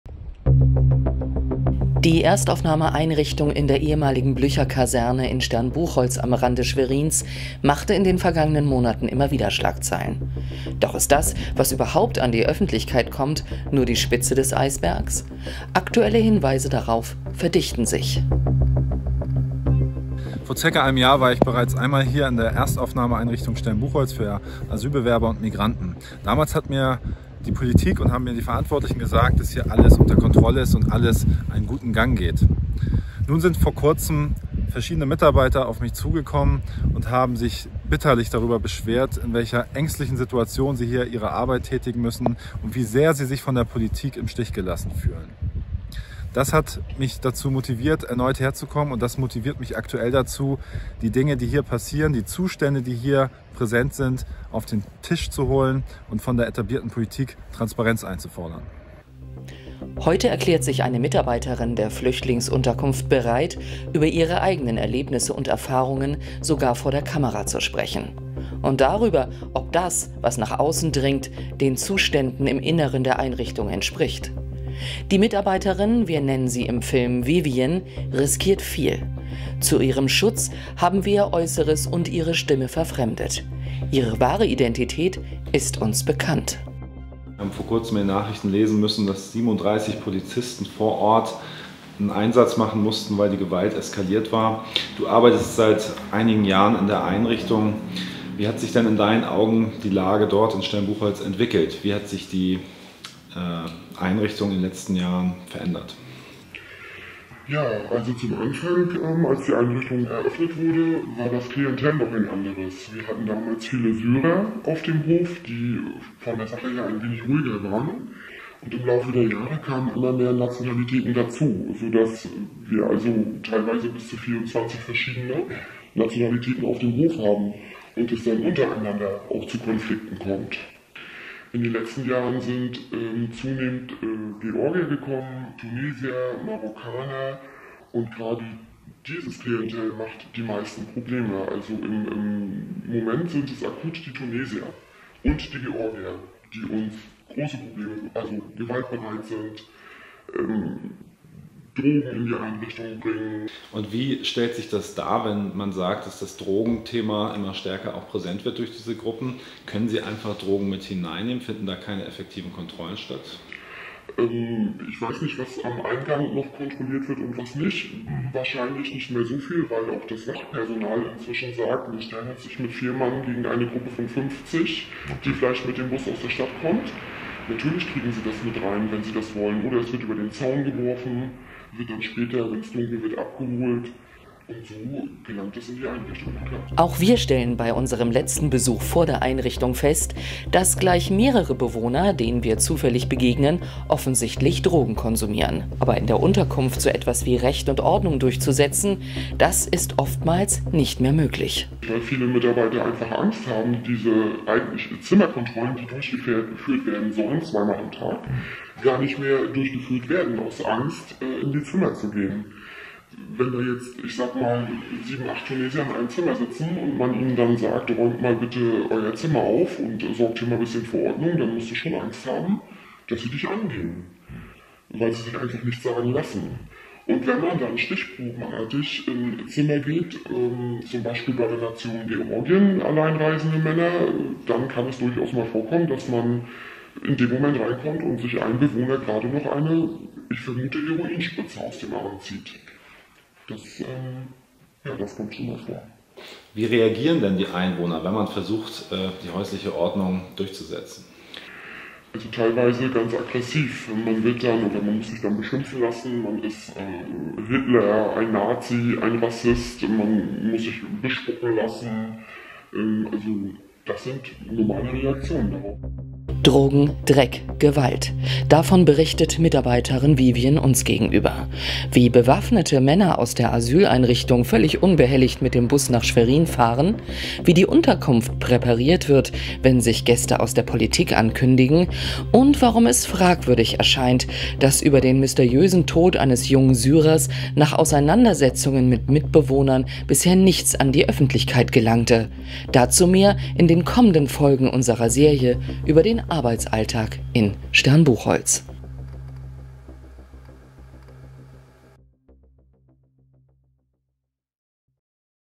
Stern Buchholz: Die bittere Realität einer Asylunterkunft - eine Mitarbeiterin packt aus! (Teil 1)
+++ Inside Stern Buchholz: Die bittere Realität in einer Asylunterkunft - eine Mitarbeiterin packt aus! (Teil 1) +++

Wir starten heute mir einer neuen Doku-Reihe über die wahren Zustände in der Erstaufnahmeeinrichtung und das Schweigen der Regierung. Dazu lassen wir exklusiv eine mutige Mitarbeiterin zu Wort kommen, die stellvertretend für viele weitere Kollegen ihr Schweigen bricht und über Drogen, Dreck und Gewalt berichtet.

Stern Buchholz geriet zuletzt immer wieder durch Schlägereien und Polizeineinsätze in die Schagzeilen der Medien. Wie schlimm die Zustände wirklich sind und wie ignorant mit den Sorgen der Mitarbeiter umgegangen wird, könnt ihr in den kommenden Tagen in mehreren Doku-Teilen selbst sehen.

Unsere Kontakte und Recherchen haben ergeben, dass Mitarbeiter der Einrichtung, die einst mit einem humanitären Selbstverständnis angetreten sind, selbst zunehmend in Angst um ihre körperliche Unversehrtheit geraten und innerlich kündigen. Die Politik der Landesregierung ist an dieser Stelle ganz offensichtlich gescheitert.